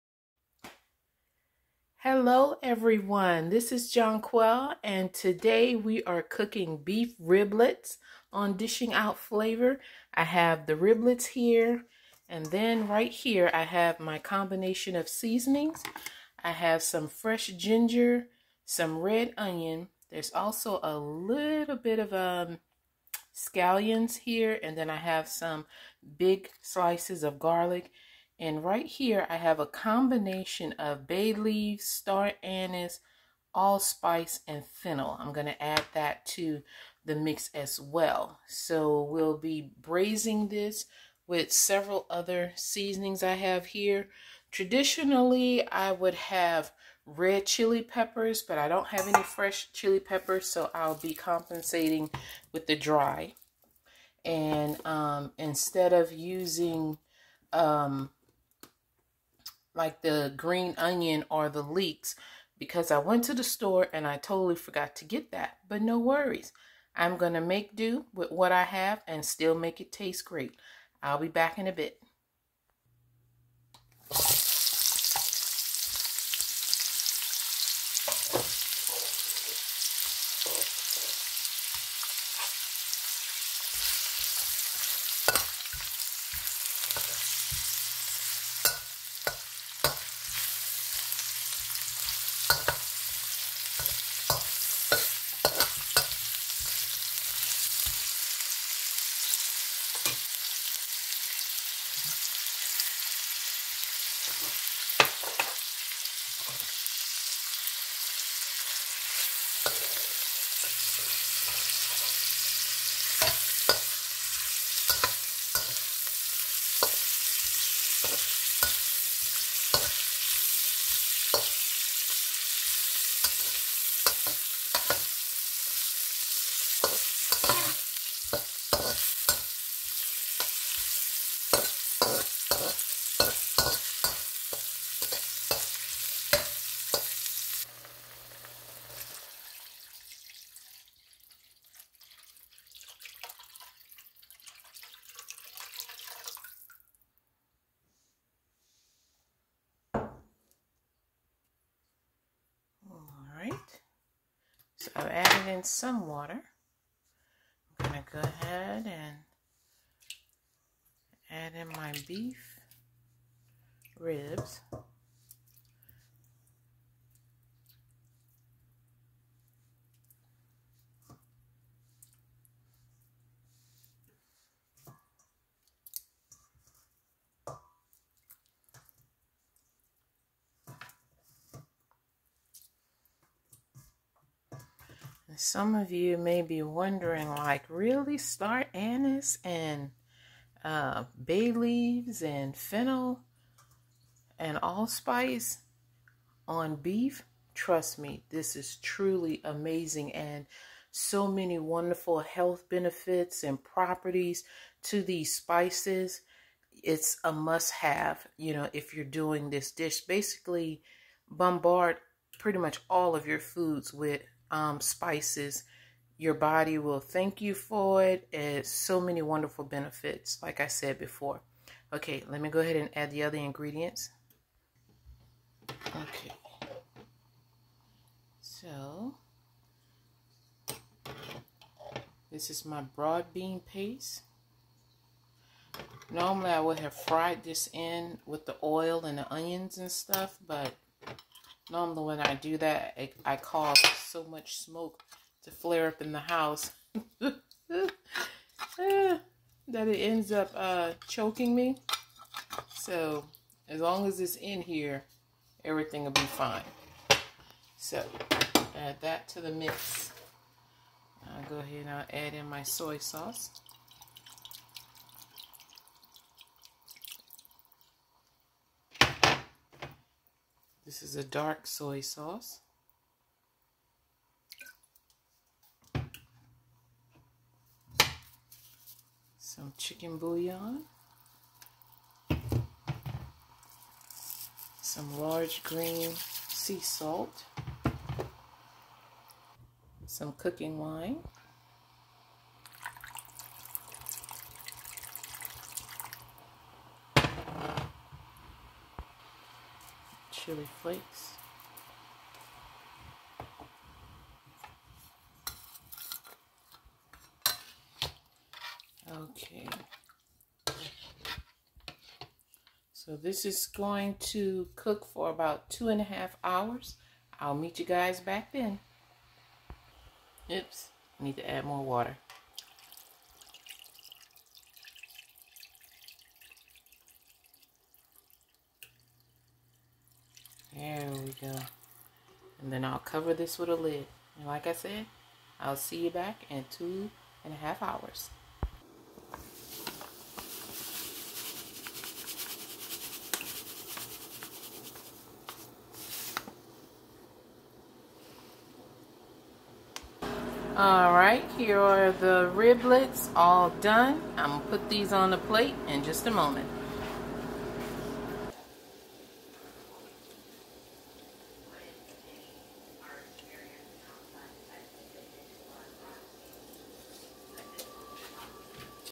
Low-carb Spicy Braised Beef Riblets
I didn't measure anything

So going by eyeballing estimates:
20-30grams red onion
20-25 grams of fresh ginger
10-12 grams of shallots
7-10 grams of garlic
2 packets of beef riblets from Walmart
5-7 grams Asian chili flakes
3 tablespoons of Broad Bean Paste
7-10 dash of low-sodium soy sauce
2 tablespoons dark mushroom soy sauce
2 teaspoons chicken boullion
2 teaspoons of large grain sea salt
1 tablespoon of Truvia

Enough water of cover the meat then cover with a lid and boil on high for 2-2.5 hours. Eat with lots of steamed green veggies.

No nutritional facts cause you'll be adding your own desired amounts of seasoning.
It was 15 beef riblet so this can serve a family of 3-5 people.